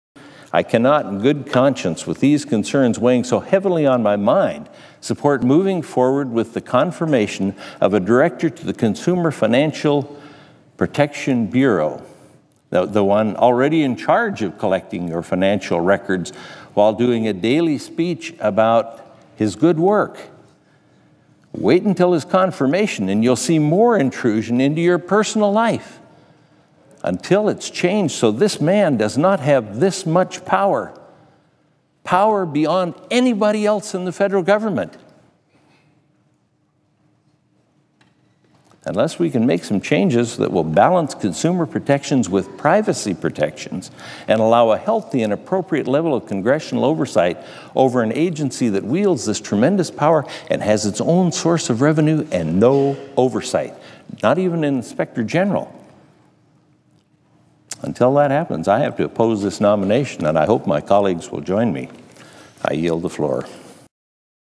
Enzi: "I cannot in good conscience"
Americans cannot tell the government they don't want their personal financial information collected or stored. That was the message U.S. Senator Mike Enzi, R-Wyo., shared when he spoke against the nomination of Richard Cordray to be the Director of the Consumer Financial Protection Bureau (CFPB) during a speech on the Senate floor today. Enzi expressed concerns with approving a director to the bureau before the lack of congressional oversight and the blatant privacy intrusions being undertaken by the CFPB are addressed. Excerpts from Enzi's speech are below.